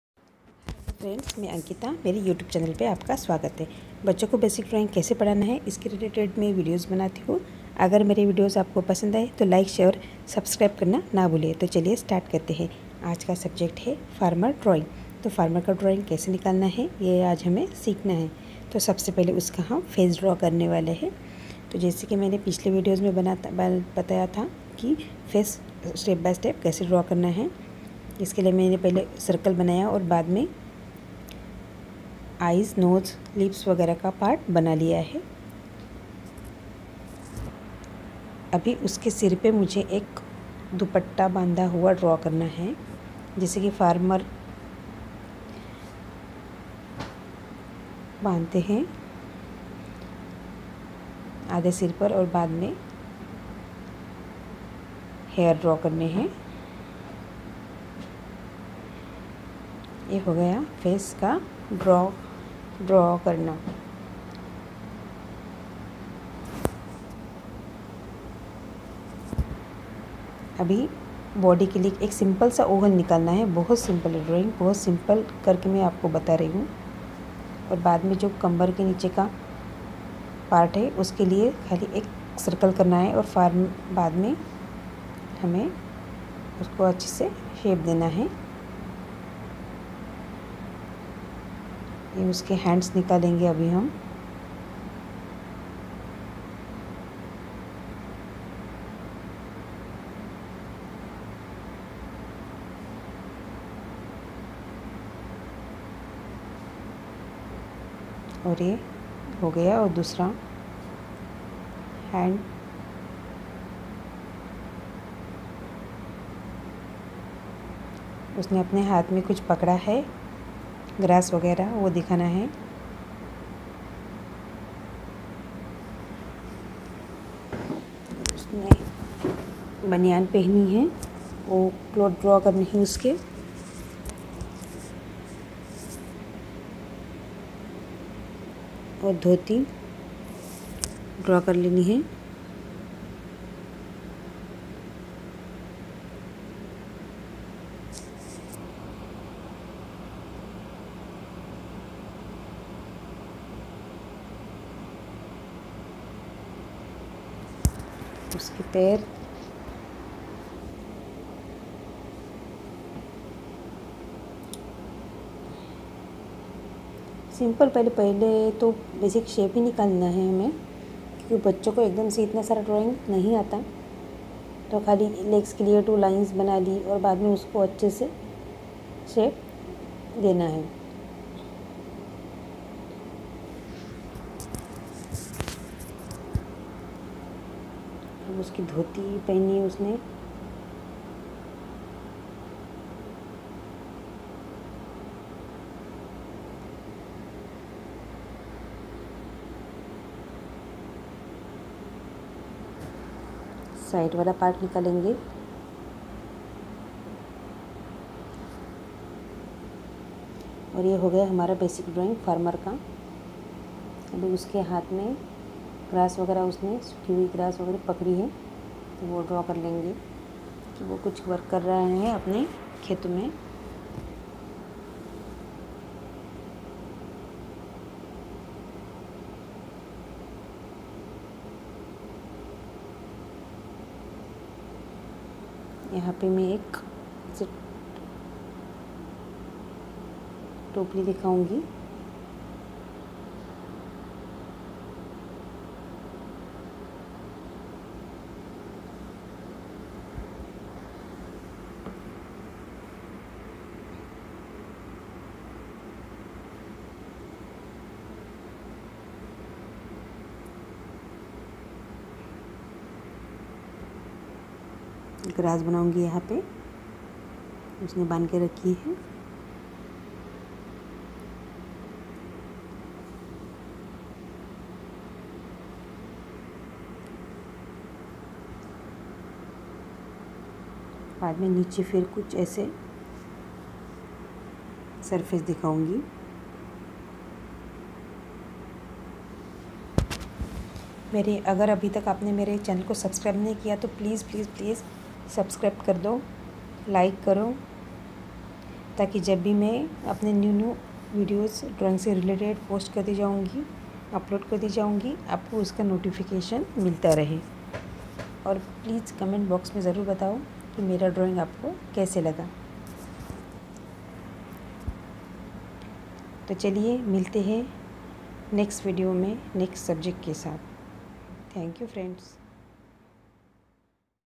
How to draw farmer drawing step by step for kids and beginner
Learn how to draw farmer  step by step drawing for kids and beginner.. Easy drawings,simpal drawing, koinsabhi drawing Ho vo simple step me kaise draw kre sikhiy ankiita k sath